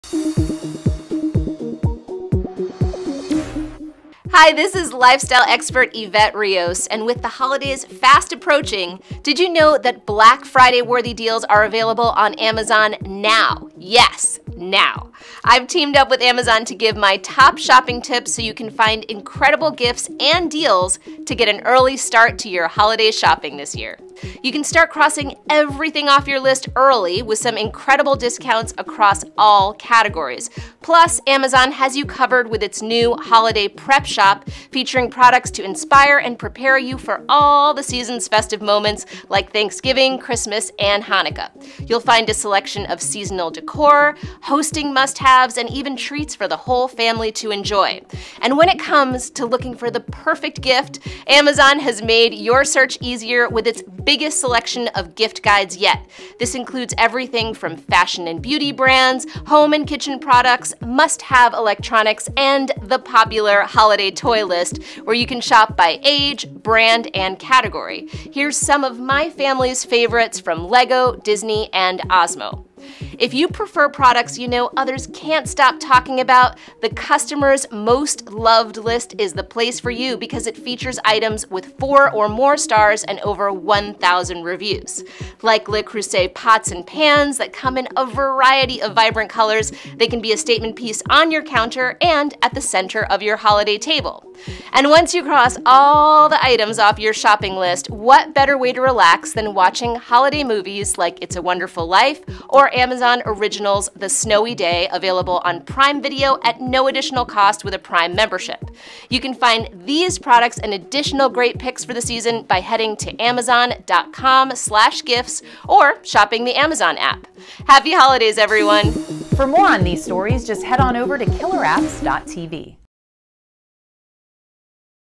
Amazon’s Early Holiday Shopping Tips with Evette Rios
Lifestyle Expert Evette Rios teamed up with Amazon to give top shopping tips to find Black Friday worthy-deals on Amazon.com/gifts now! You can start crossing everything off your list EARLY with some incredible discounts!